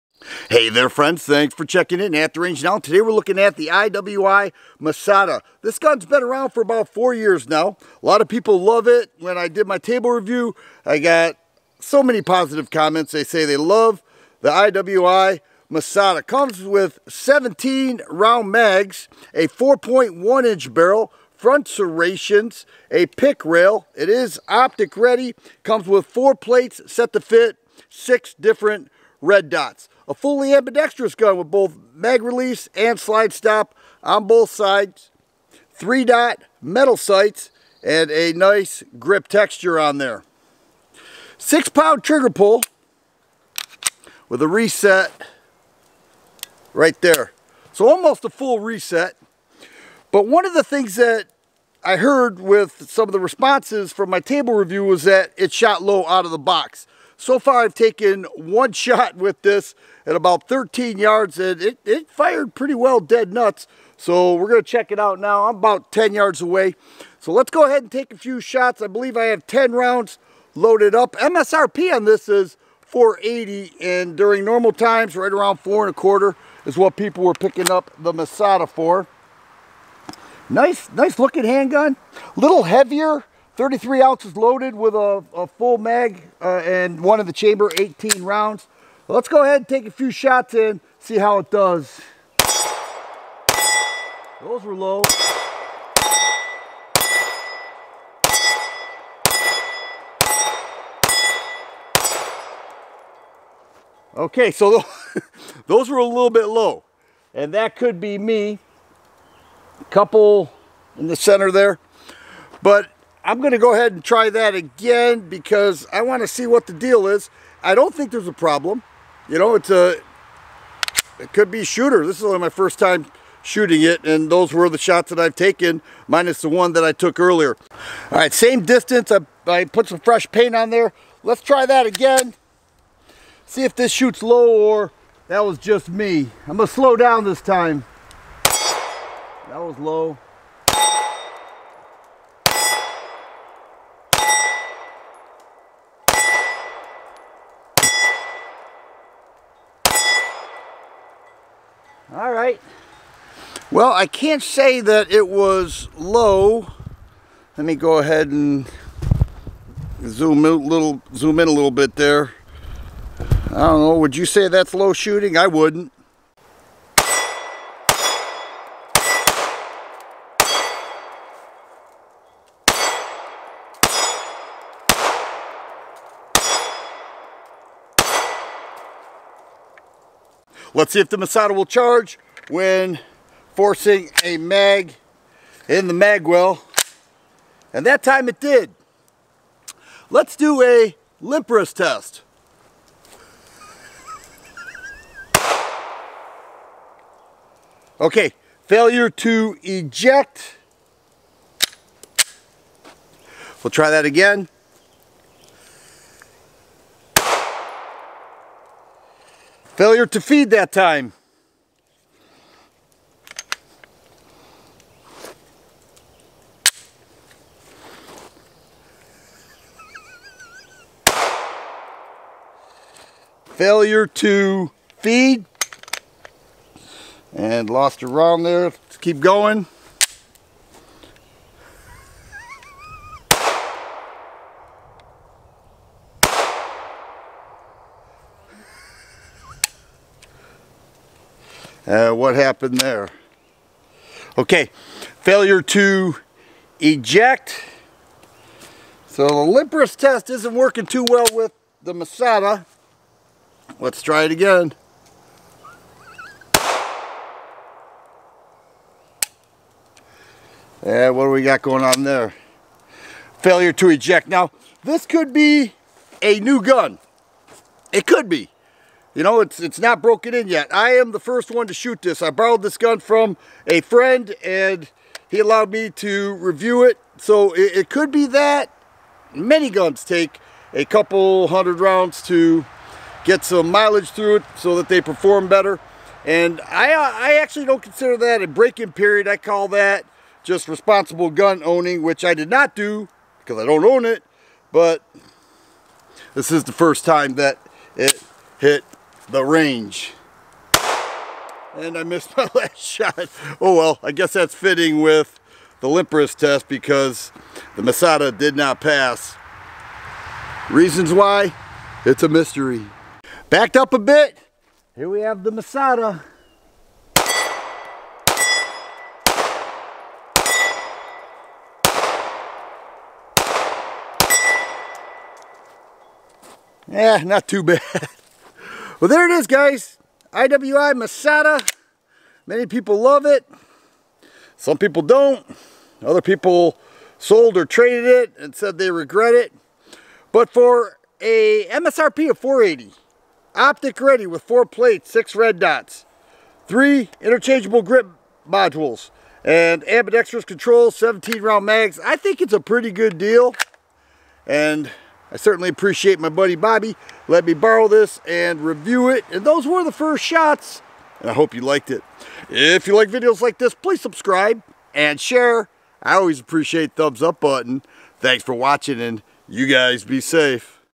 Masada Range Review with Limp Wrist Test - TheFirearmGuy
Here is an IWI Masada Range Review with a "limp wrist" test. The Masada offers a lot for the money. It is optic ready, ships with two 17 round mags and has many features that shooters enjoy. Check it out and let me know your thoughts.